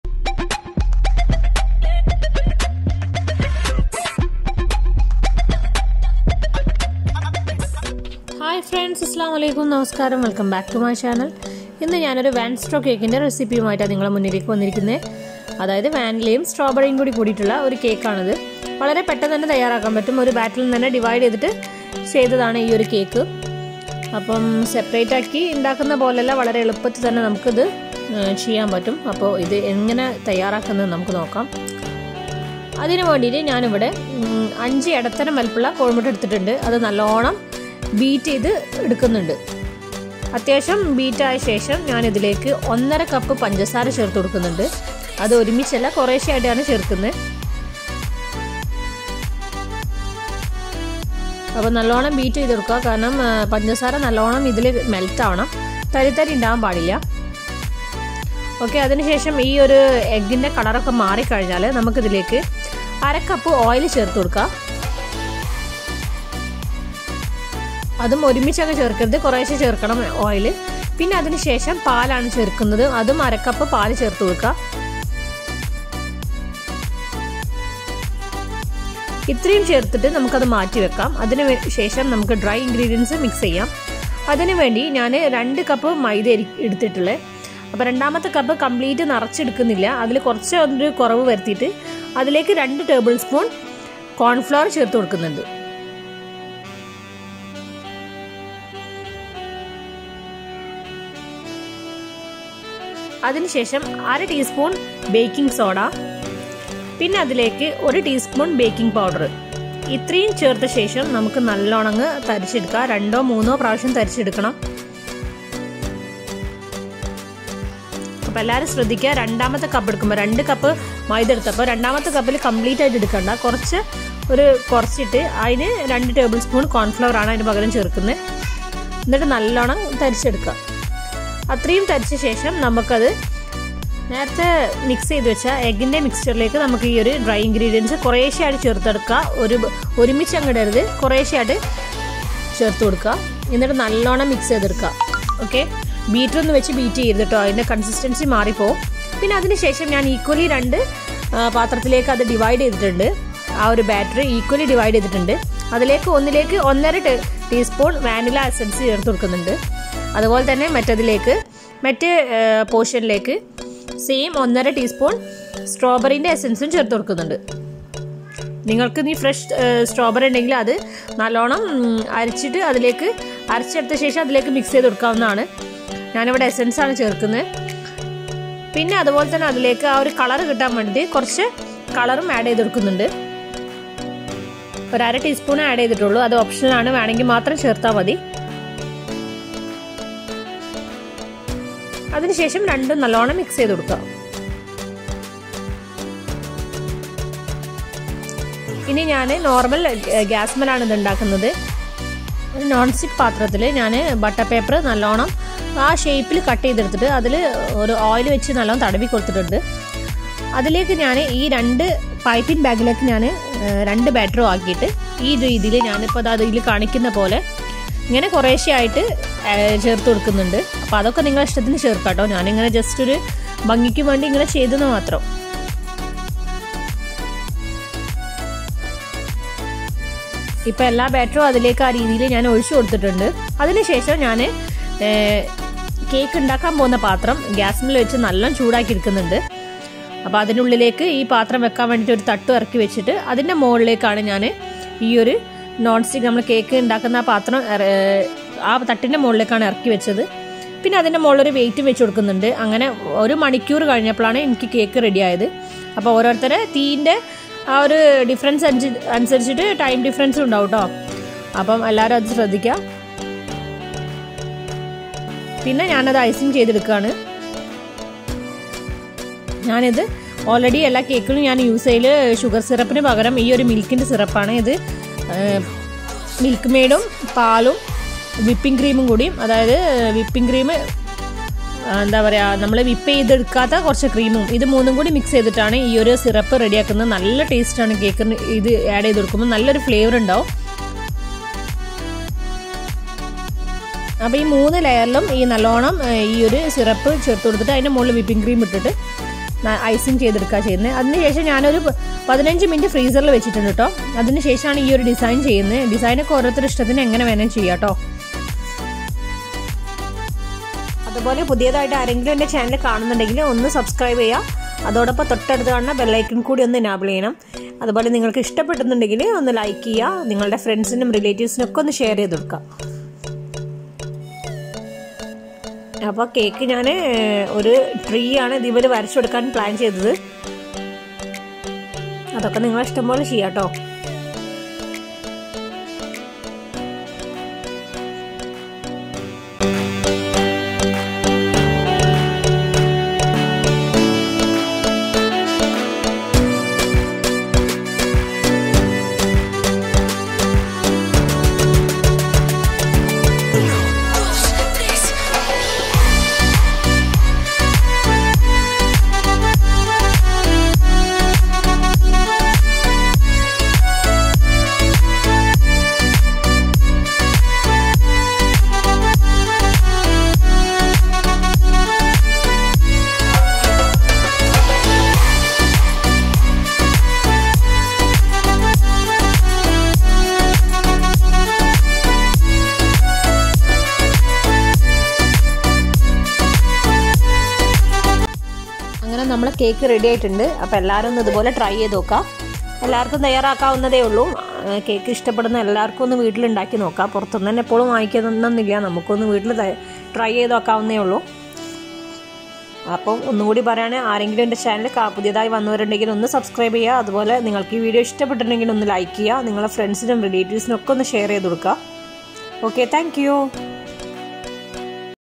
Vanstraw cake/vanilla strawberry cake/Christmas cake
Maida-2cups+2tbsp cornflour
Egg-5
Oil-1/2 cup
Milk-1/2cup
Vanilla essence-1 1/2tsp
Strawberry essence-1tsp

Musician: ASHUTOSH